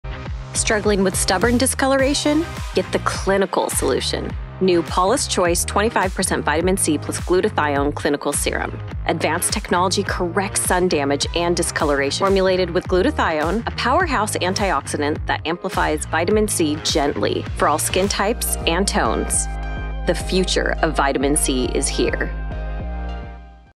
25% Vitamin C + Glutathione Clinical Serum
Meet the clinical solution for stubborn discolouration & sun damage – our 25% Vitamin C + Glutathione Clinical Serum.

Featuring highly stable 25% Vitamin C and skin-soothing Glutathione to visibly improve uneven tone, radiance & firmness.